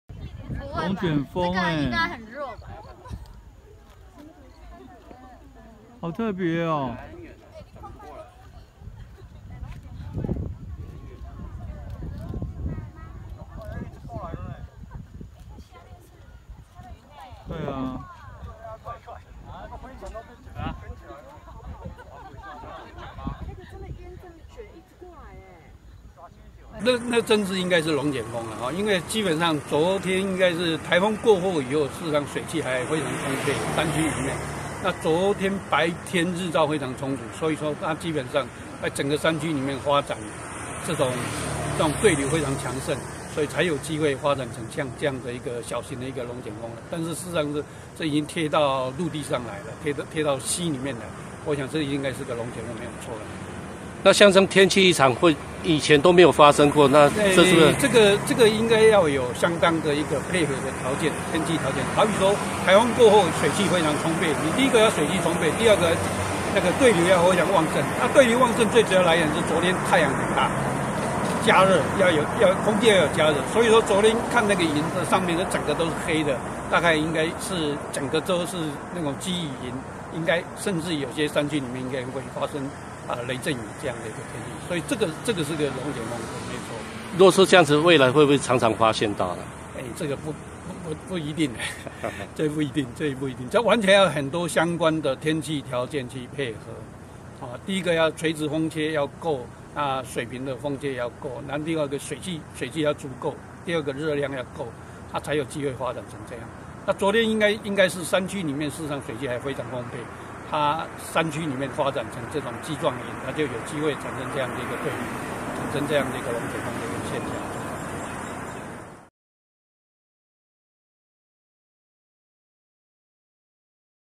六十石山花海拍到龍捲風！農民採金針遇罕見奇景(遊客提供)
富里鄉六十石山正值金針花季，富里鄉金針農民張秀發在line群組貼出一張「金針花海龍捲風」奇景照片，照片視角是從海拔900公尺的六十石山上往下俯瞰花東縱谷，天空烏雲密佈，從空中伸下一條細細長長漏斗狀龍捲風，前景還有金針花海！